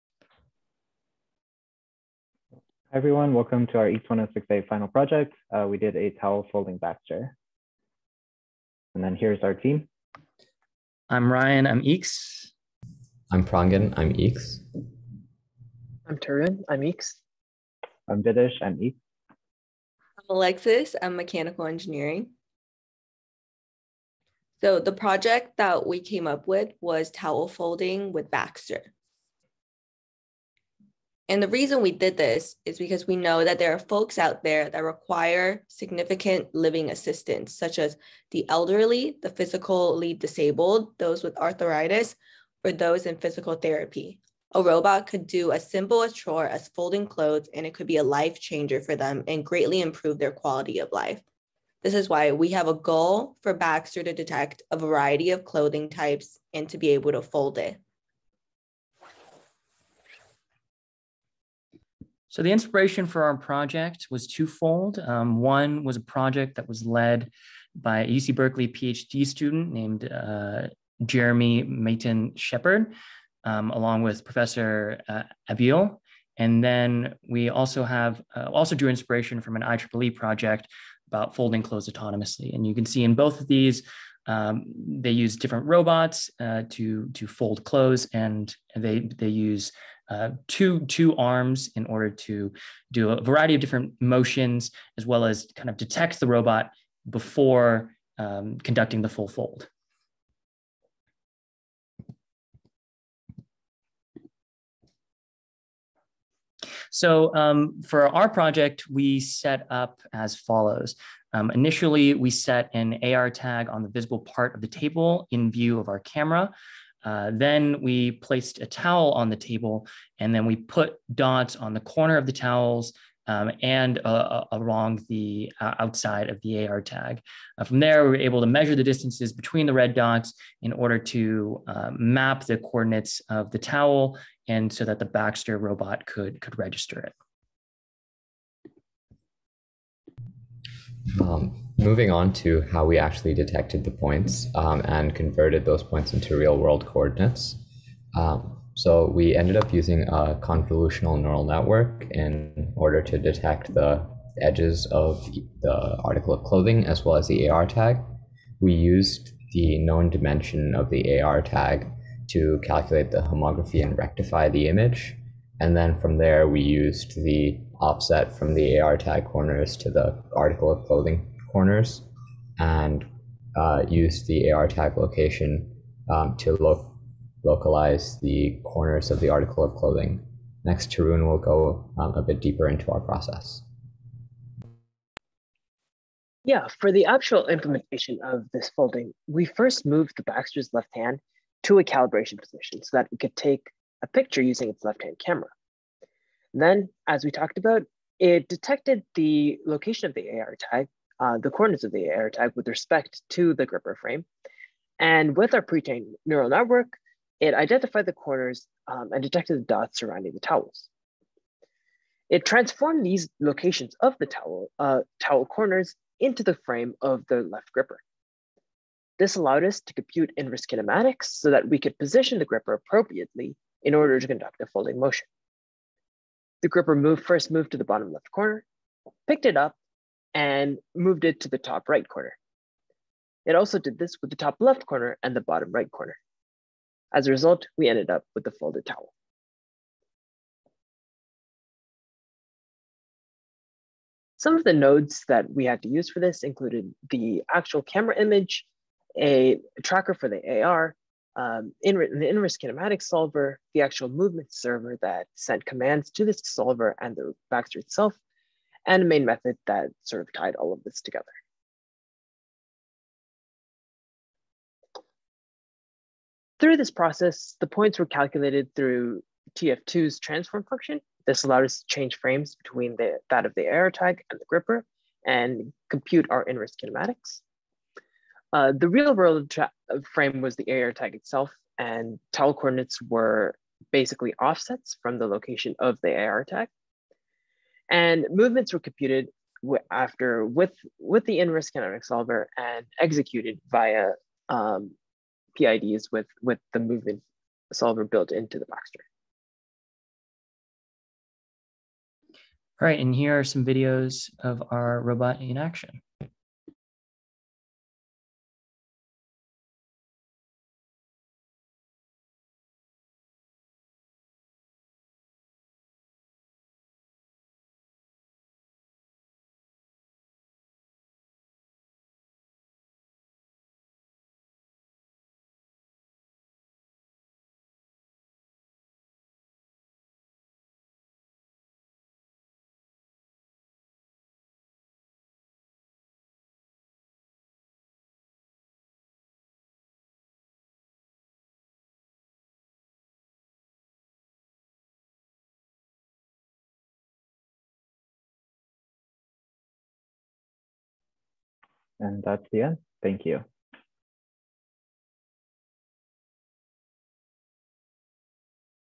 Towel Folding Baxter Overview Video
Overview video for the EECS 106A Fall 2021 Class! Our project was a Towel Folding Baxter.